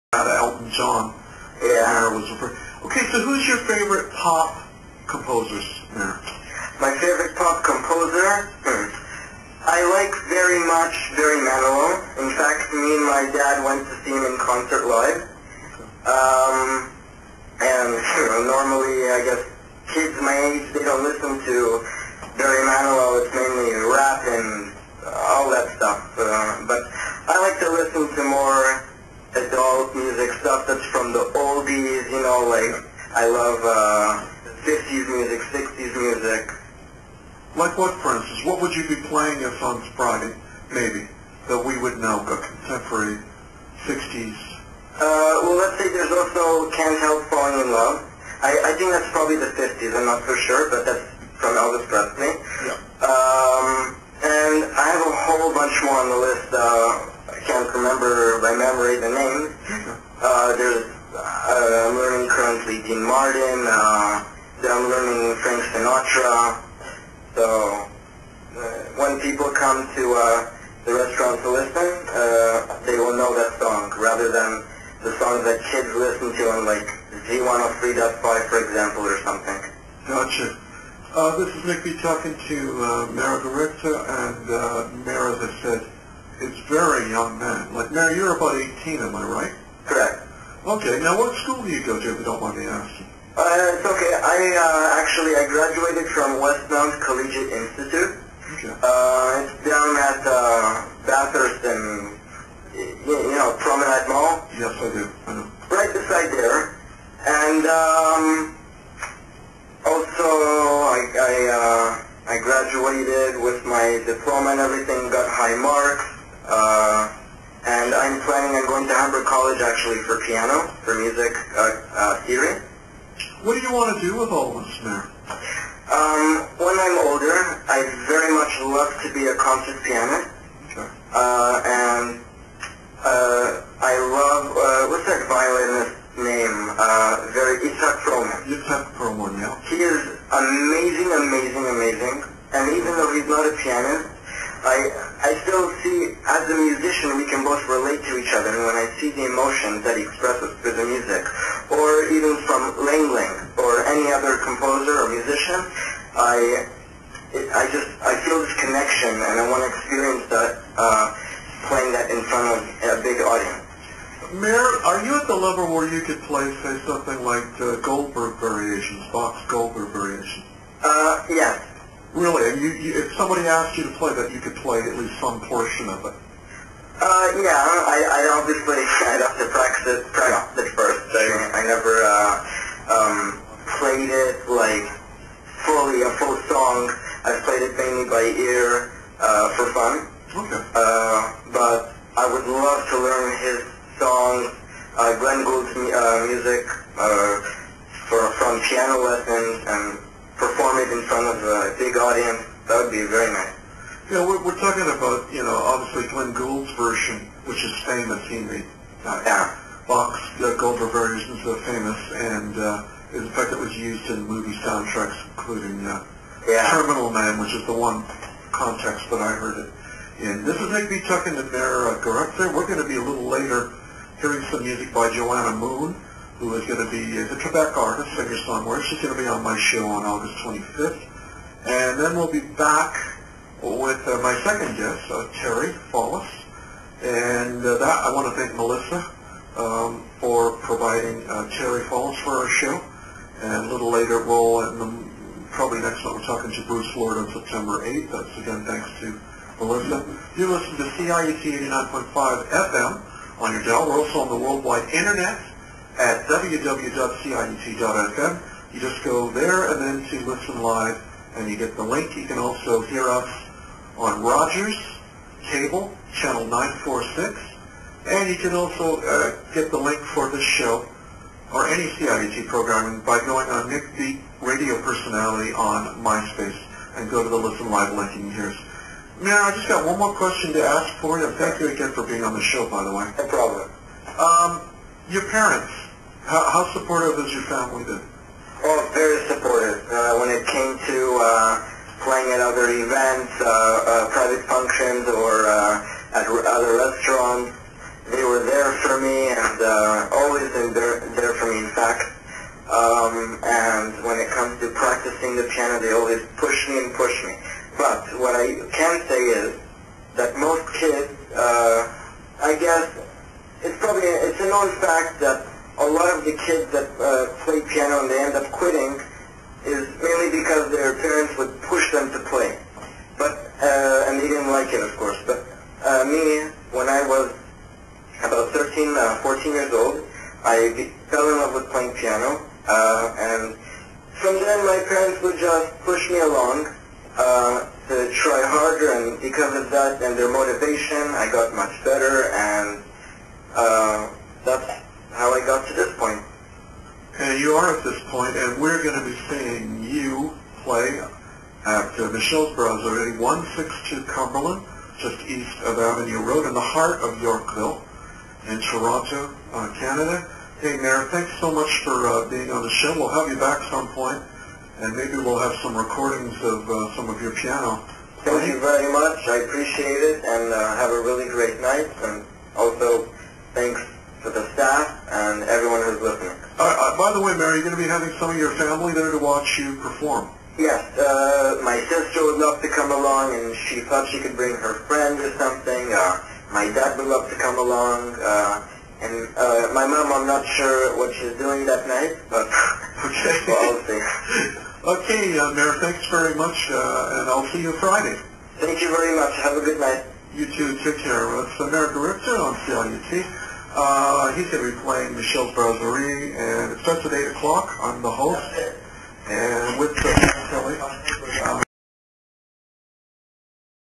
(2) Meir Gerichter The Pianist- Live @ CIUT Radio89.5 FM Interview!
Hey Youtubers! this video is mainly about the great success that I've been having with my Piano/Music Career as an 18 year old. Hosting CIUT Radio (Also known as University of Toronto Radio) is Nick Beat. Thank you very much, enjoy!!!